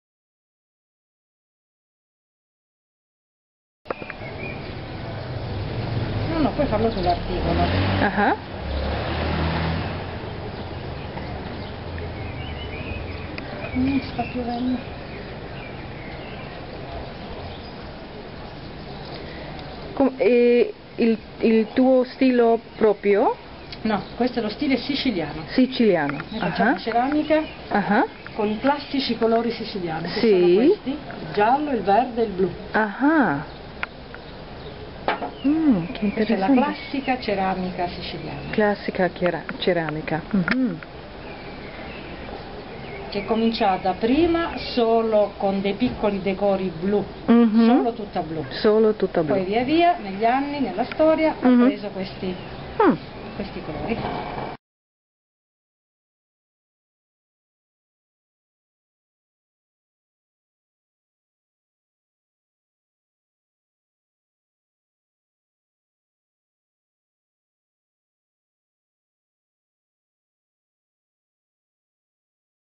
Adventures in Sicily: Castelmola, Taormina, Isola Bella 18
During my language study trip in Taormina, Sicily, I enjoyed several local excursions. Together with my language study colleagues from the Babilonia Language School I hiked to the top of Castelmola. I also explored Isola Bella, a beautiful stretch of Mediterranean coastline at the foot of Taormina.

For articles about unconventional travel and cross-cultural connections feel free to visit my